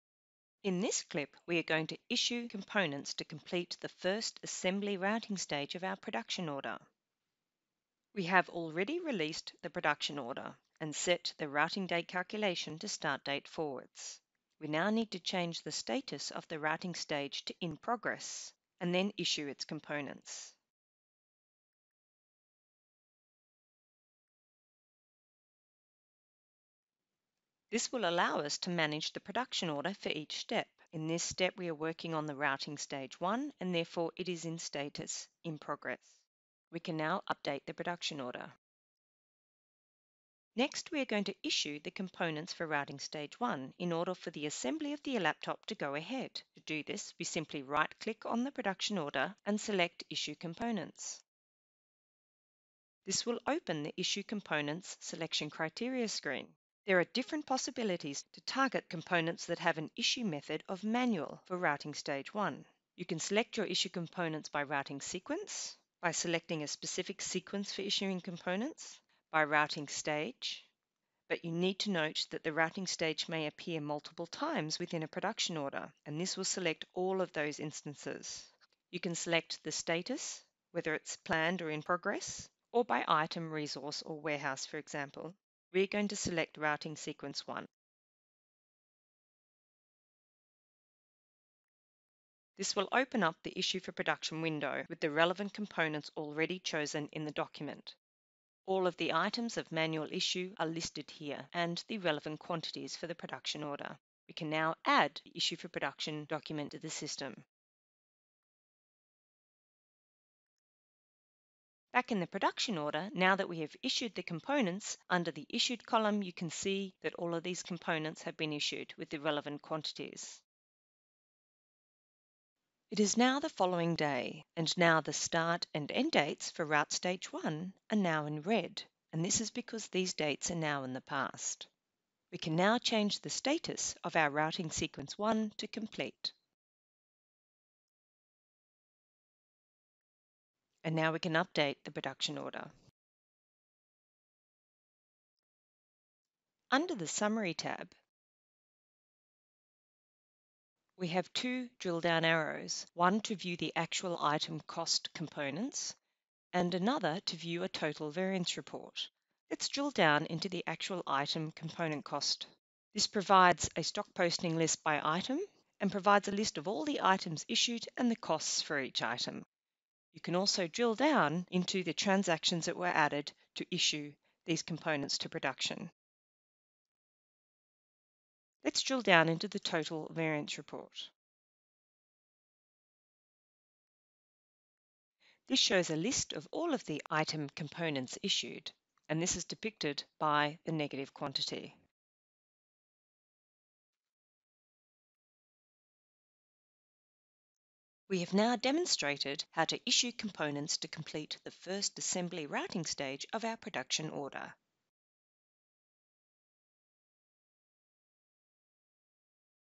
SAP Business One - Issuing Components & Completing Route Stage 1
In this clip, we are going to issue components to complete the first Assembly Routing stage of our production order.